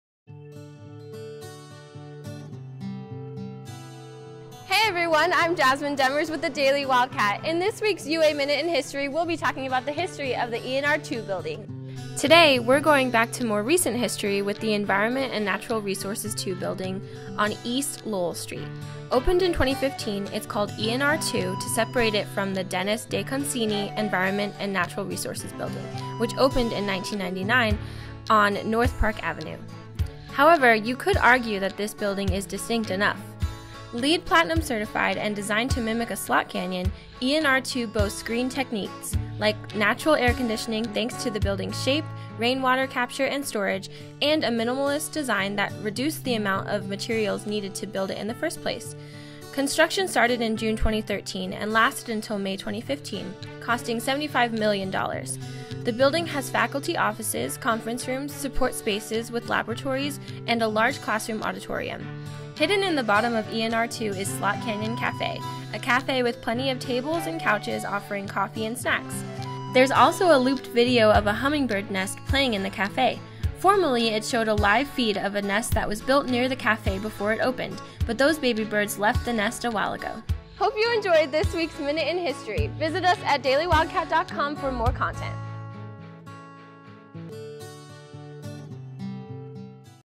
UA Minute in History: ENR2
What's the story behind the design of ENR2? Find out with Editor in Chief Jasmine Demers in this UA Minute in History.

Video by Marissa Heffernan and Priya Jandu